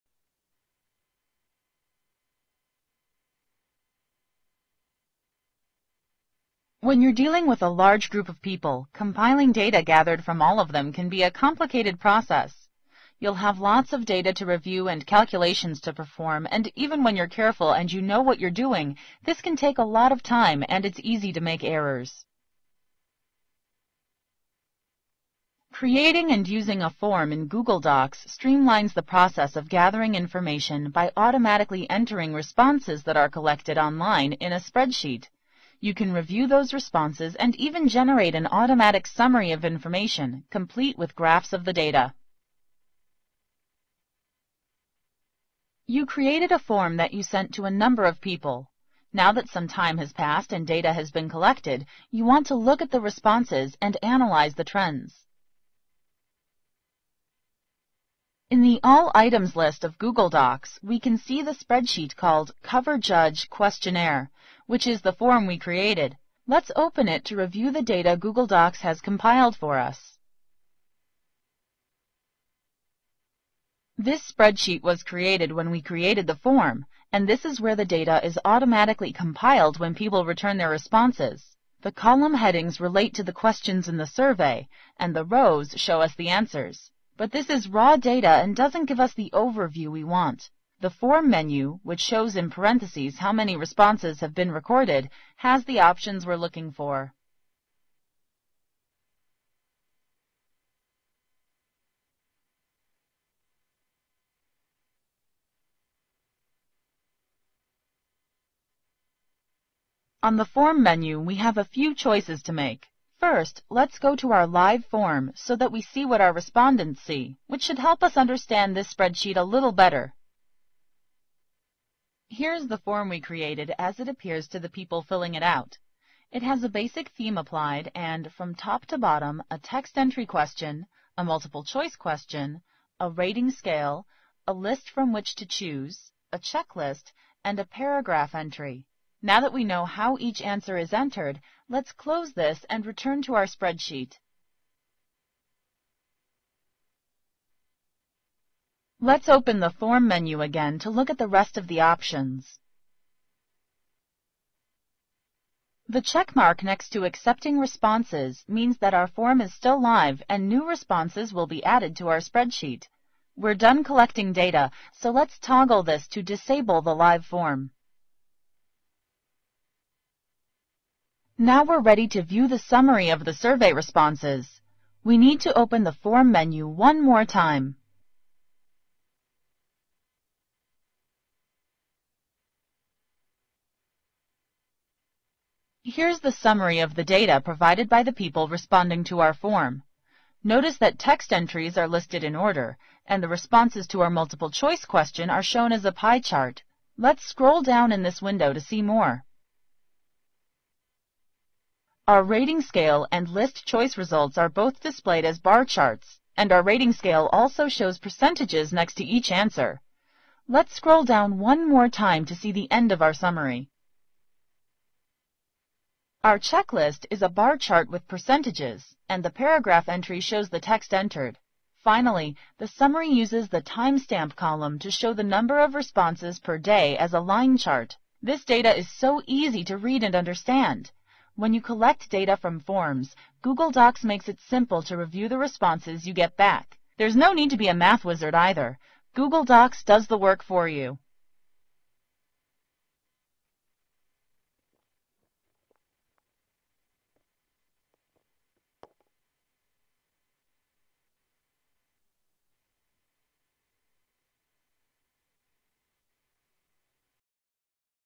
Google Forms - Collect Results from a Form - Google Tools
Discusses how Google compiles responses to a form. Shows how to access form results as a summary and as a spreadsheet.